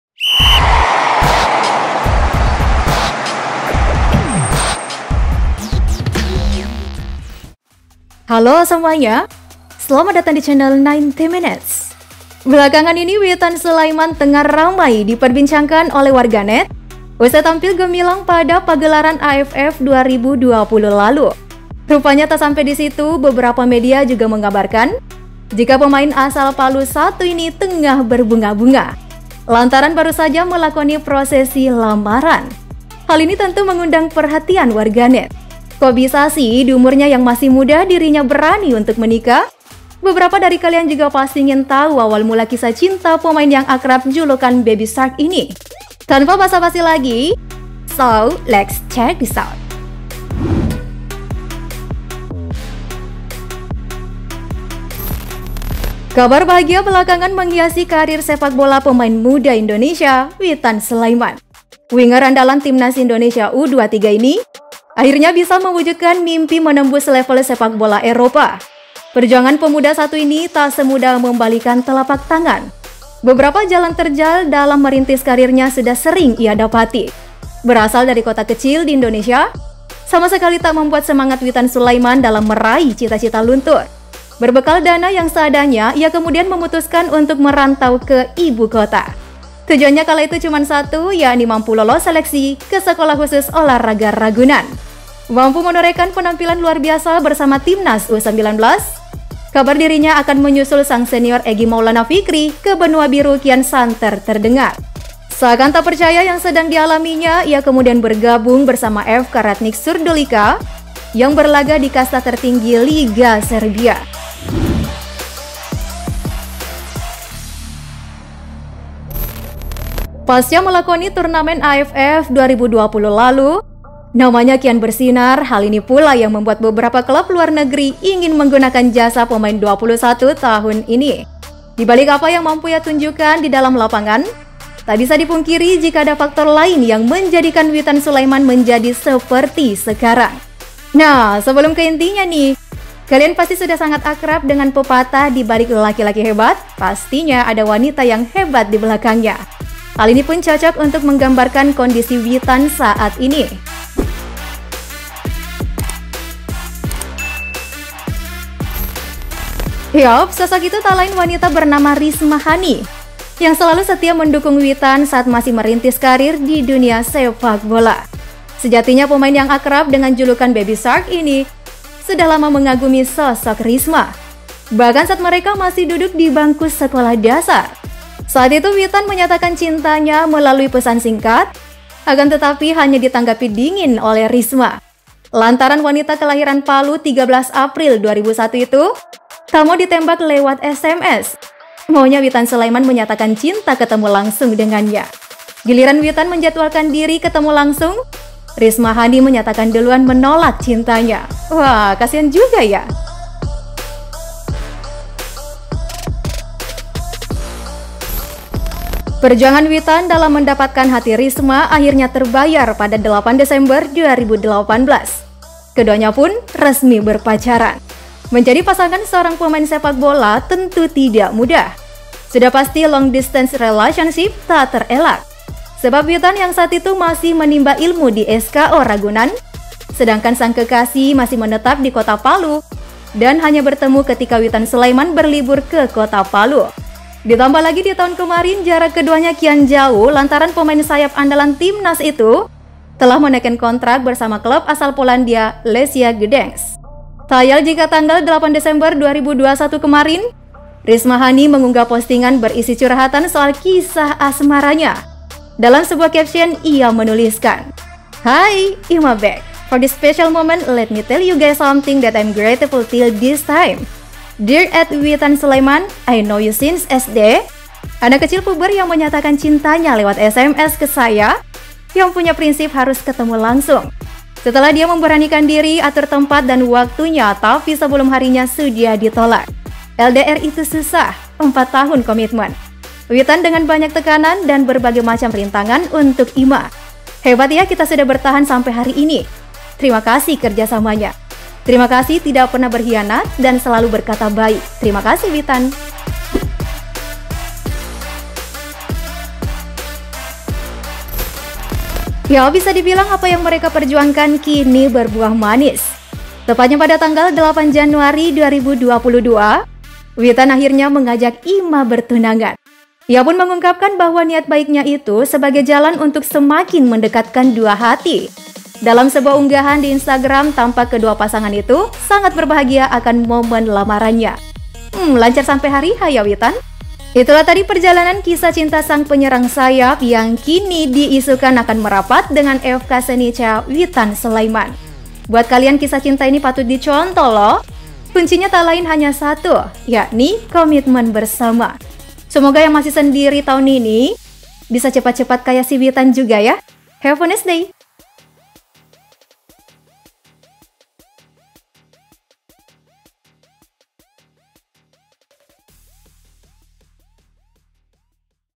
Sukses Bikin Netizen Iri! Begini Kisah Cinta Witan Sulaiman Dengan Rismahani
Belakangan ini Witan Sulaiman tengah ramai diperbincagkan oleh warganet usai tampil gemilang pada pagelaran AFF 2020 lalu. Rupanya tak sampai disitu, beberapa media juga mengabarkan jika pemain asal Palu satu ini tengah berbunga bunga lantaran baru saja melakoni prosesi lamaran. Hal ini tentu mengundang perhatian warganet kok bisa sih, diumurnya yang masih muda dirinya berani untuk menikah ? beberapa dari kalian juga pasti ingin tahu awal mula kisah cinta pemain yang akrab julukan baby Shark ini. Tanpa basa basi lagi nih, So lets check this out.
Kabar bahagia belakangan menghiasi karier sepak bola pemain muda Indonesia, Witan Sulaeman. Winger andalan Timnas Indonesia U-23 ini, akhirnya bisa mewujudkan mimpi menembus level sepak bola Eropa. Perjuangan pemuda satu ini tak semudah membalikkan telapak tangan. Beberapa jalan terjal dalam merintis karirnya sudah sering ia dapati, berasal dari kota kecil di Indonesia sama sekali tak membuat semangat Witan Sulaiman dalam meraih cita cita luntur, berbekal dana yang seadanya ia kemudian memutuskan untuk merantau ke ibu kota. Tujuannya kala itu cuman satu yakni mampu lolos seleksi ke sekolah khusus olahraga Ragunan. Mampu menorehkan peanmpilan luar biasa bersama timnas U-19, kabar dirinya akan menyusul sang senior Egy Maulana Vikri ke benua biru kian santer terdengar. Seakan tak percaya yang sedang dialaminya, ia kemudian bergabung bersama FK Radnik Surdulica yang berlaga di kasta tertinggi liga Serbia.